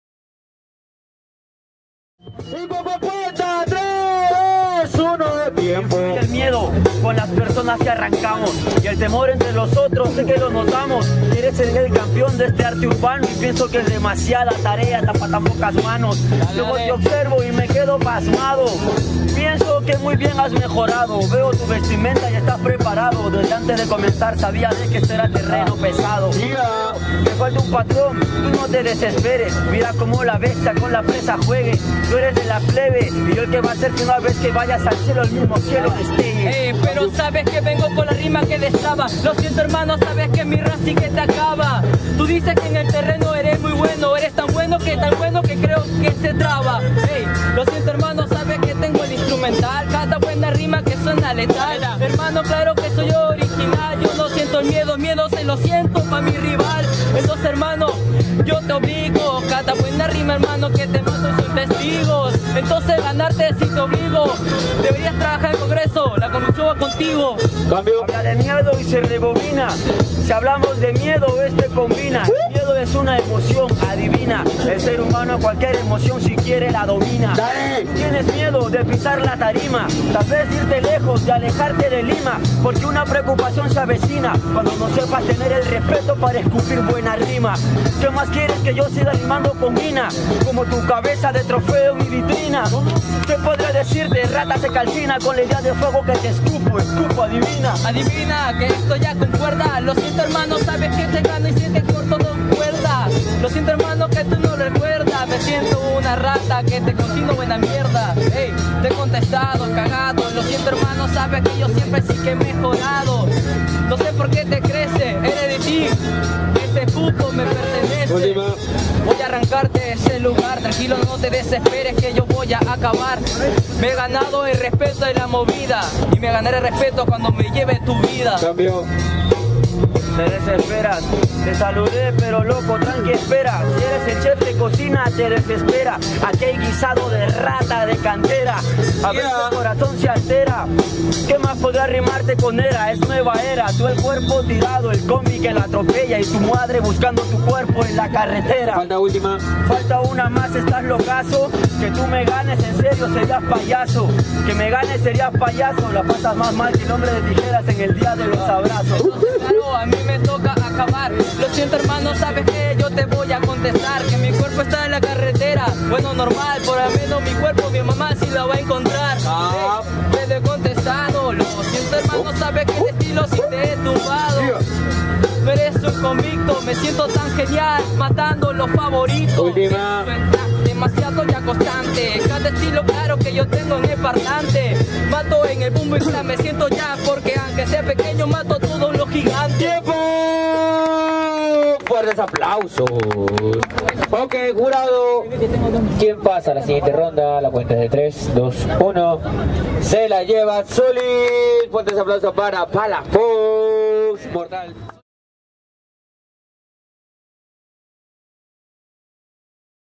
SOLID vs PALAFOX - Cuartos - P09 Battles - Fecha 14
Colectivo P09 Frente al Gallo mas Gallo VMT 🐔
(Av.Pista Nueva c/ Av. Villa Maria)

Host: Jorge MC ⭐
Jueces: FAC MALCON ⭐ / LKM ⭐ / SALSERIN ⭐
Organizador: Arma MC y DJ Bot ⭐
Camara: Zonyerval Films  📸
Sonido: DJ M10 🤖

14-06-19